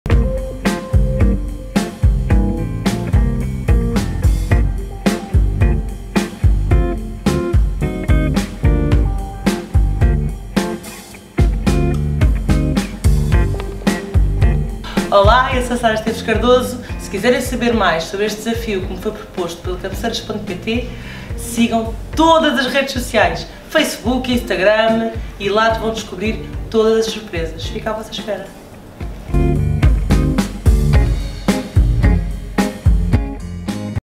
Embaixadora Cabeceiras.pt | Sara Esteves Cardoso - parte 1
A Sara Esteves Cardoso acompanhou de perto o desenrolar da sua ideia de produto! Curiosos?

Fiquem atentos e acompanhem tudo por aqui!